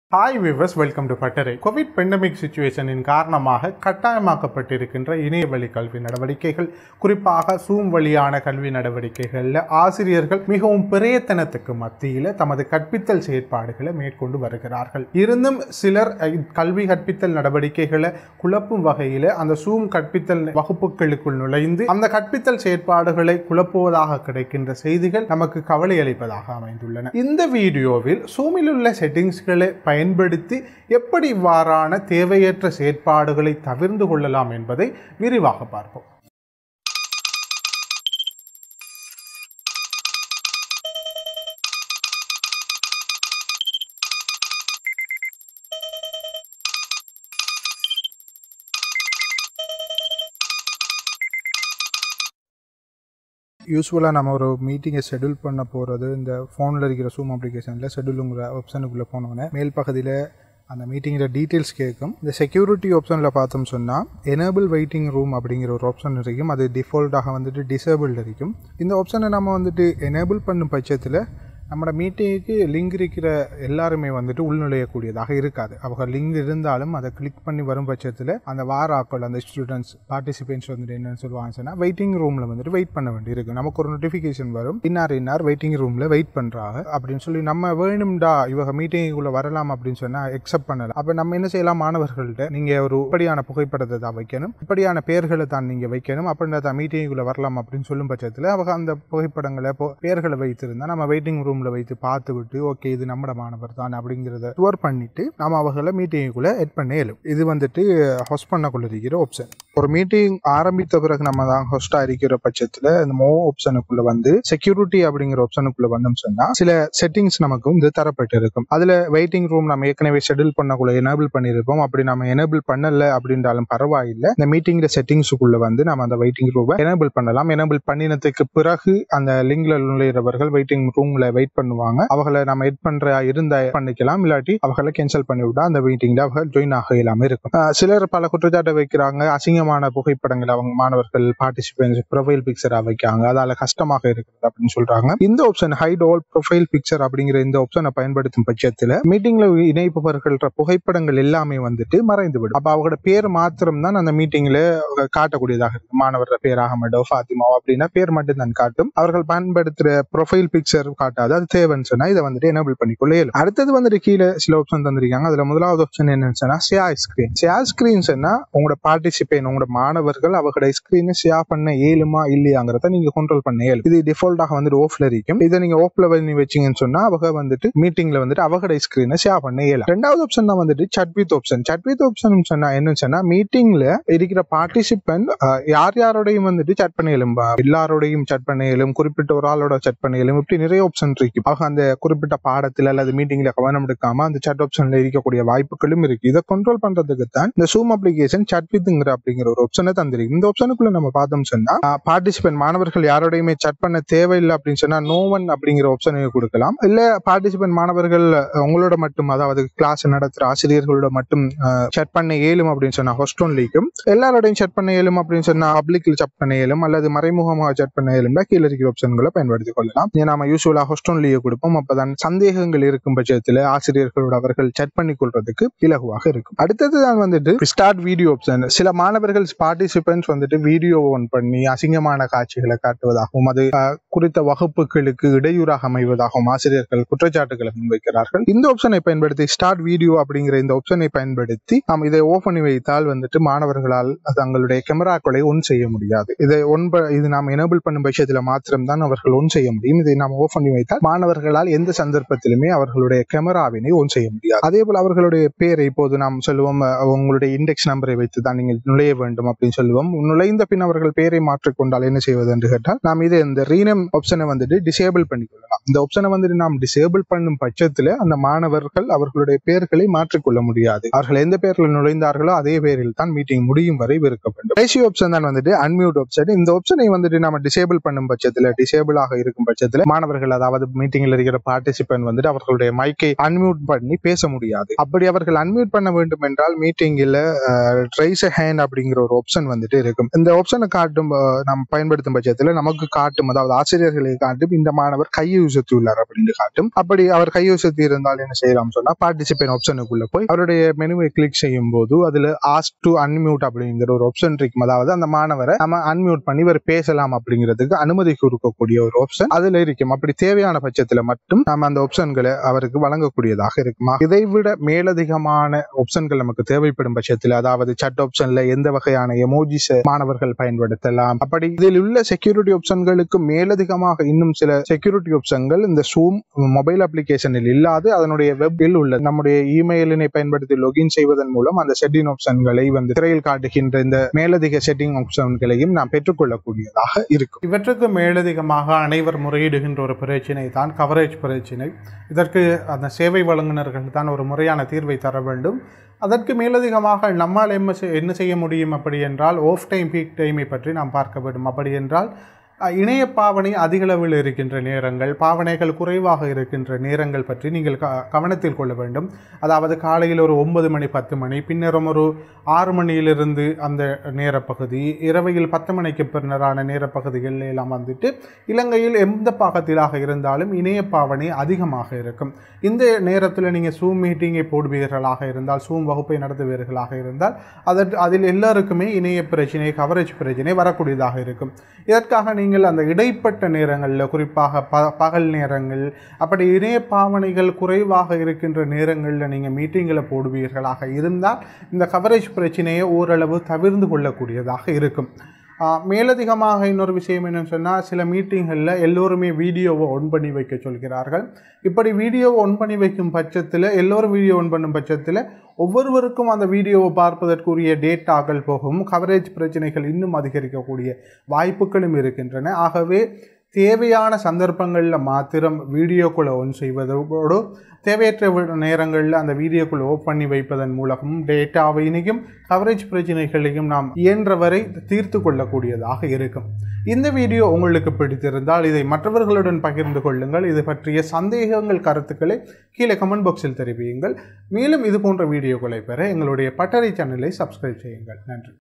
8 Security Settings in Zoom | Zoomஇல் இடையூறுகளை தடுப்பது எப்படி ?
Zoomஊடாக கற்பித்தல் நடவடிக்களை தொடர்வது தவிர்க்க முடியாமல் ஆக்கப்பட்டுள்ள நிலையில் பலர் Zoom Classஇல் நுழைந்து குழப்படிகளை ஏற்படுத்துவதாக கிடைக்கப்பெறும் தகவல்கள் கவலையளிக்கின்றன. அவற்றை இலகுவாக தவிர்க்கும் முறையினை விளக்குகின்றது இந்த வீடியோ!

This video explains security settings in Zoom App to avoid intruders who are making troubles in Zoom classes and meetings.